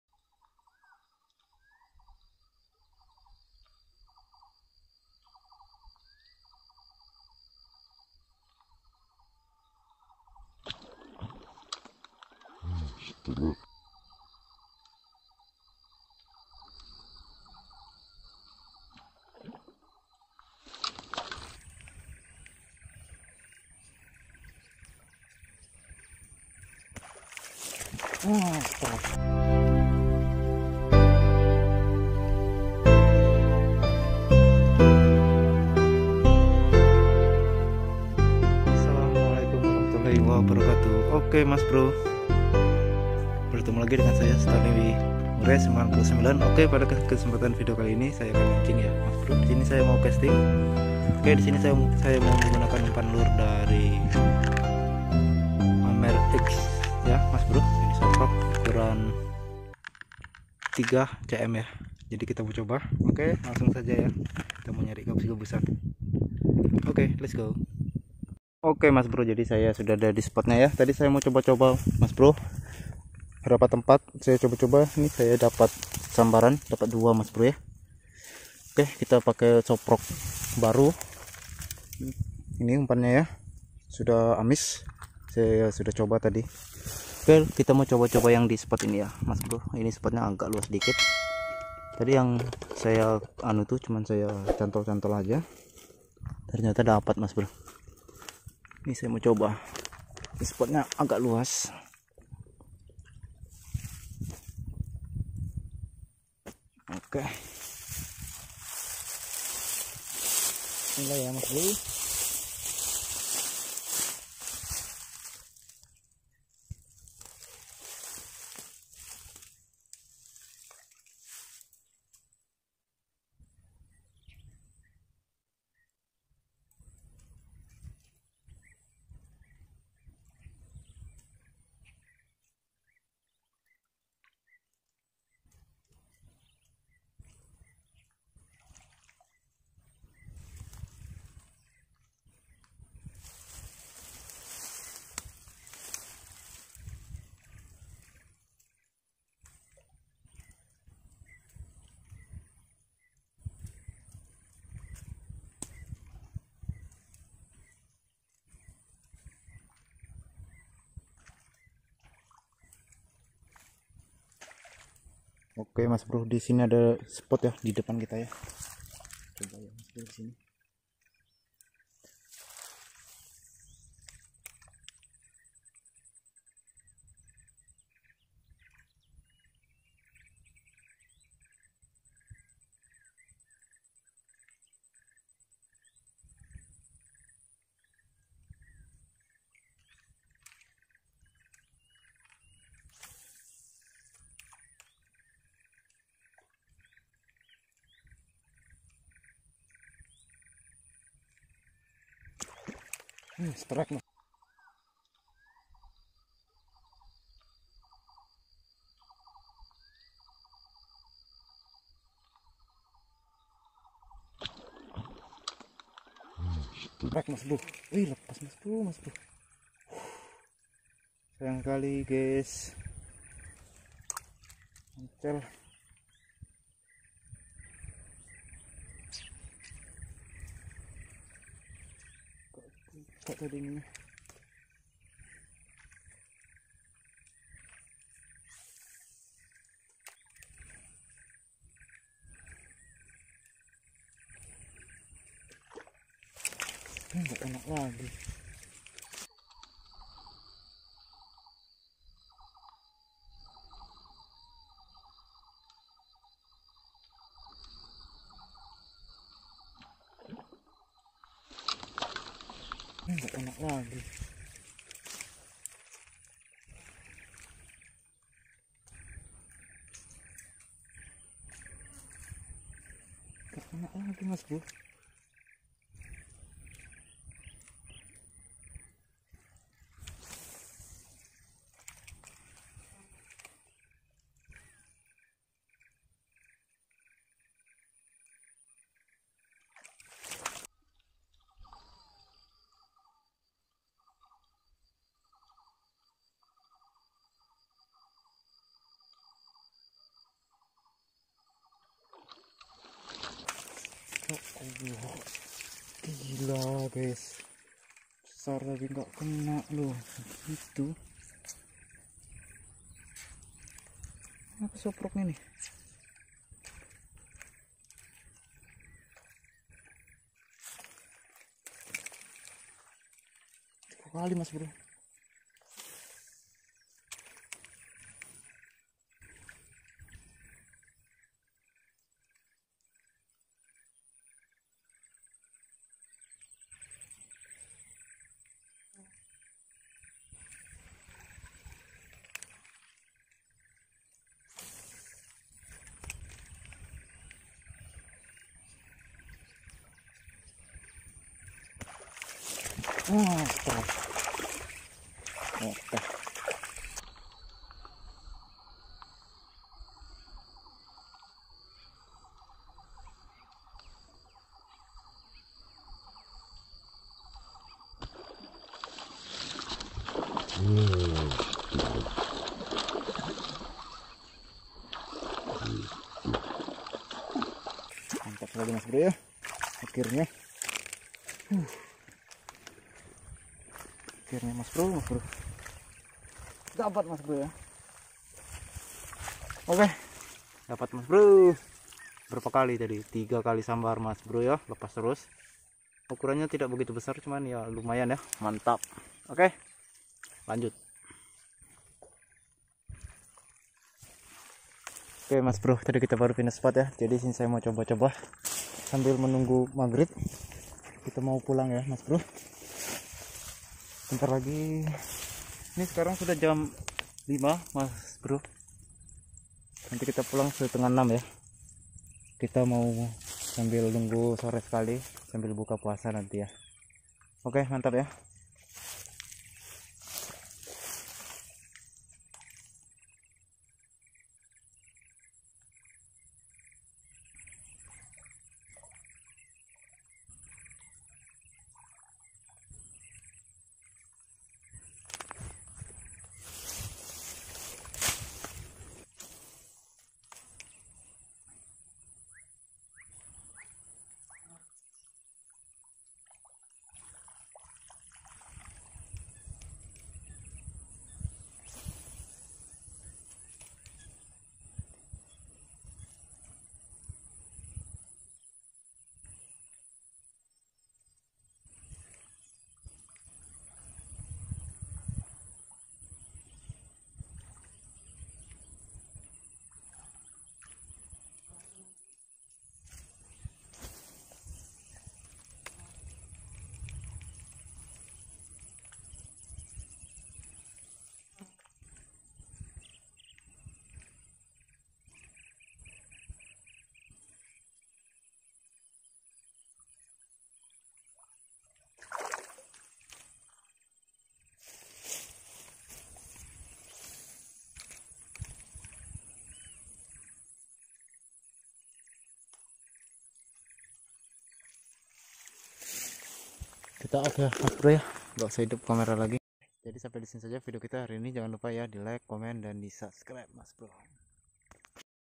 MANCING IKAN GABUS DI WAKTU BULAN PUASA || CASTING GABUS
MANCING IKAN DI WAKTU BULAN PUASA || TIDAK TERASA UDAH SORE, MANCING CASTING IKAN GABUS  SM-84

HELLO MAS BRO ...

Di Video kali ini Saya Sedang mancing Casting ikan Gabus.di sungai yang sedang Kering bayak di huni ikan gabus dan saya jga menggunakan umpan (soffrog)
Semoga video ini bisa membuat teman" terhibur amin"🙏
SET PERALATAN ...
ROD / JORAN PANCING : HIKARU PRO 12 LBS
REEL : DEUKIO 2000
UMPAN : SOFFRONG 3,5CM
KAMERA : HP REALME 5 PRO